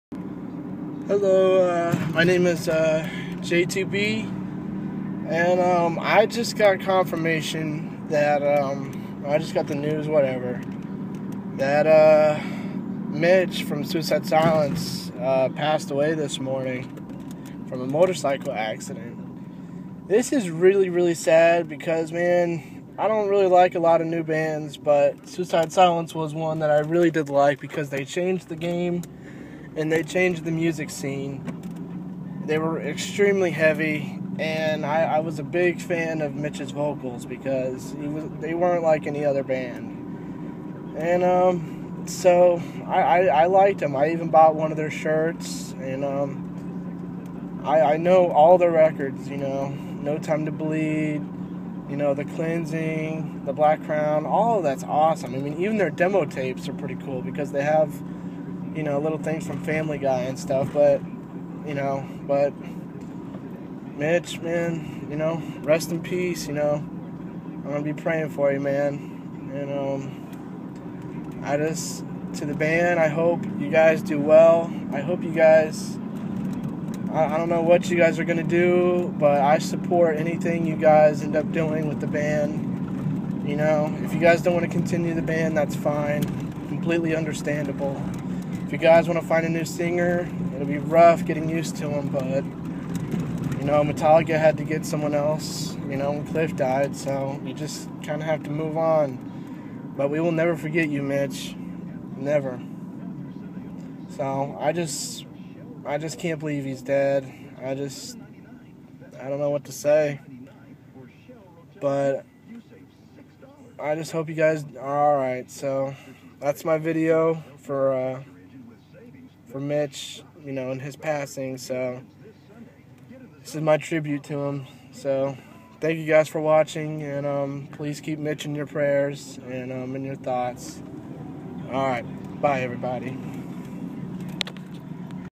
RIP MITCH LUCKER FROM SUICIDE SILENCE 2012
This is so sad to say that Mitch has passed away today :(